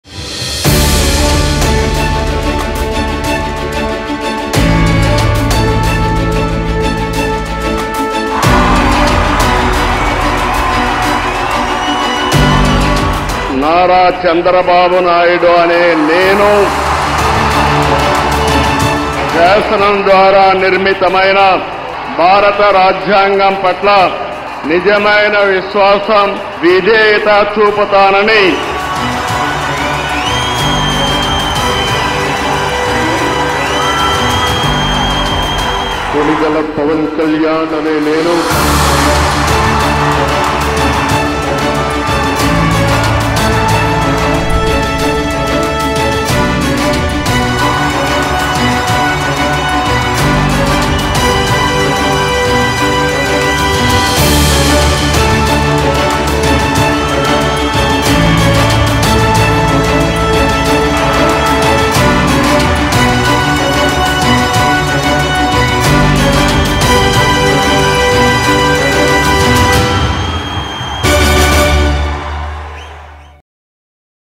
Some glimpses from the swearing-in ceremony of the new Andhra Pradesh government
Prime Minister Narendra Modi attended the swearing-in ceremony of new Andhra Pradesh government. Shri N. Chandrababu Naidu has been sworn in as the Chief Minister and Shri Pawan Kalyan has been sworn in as the Deputy CM.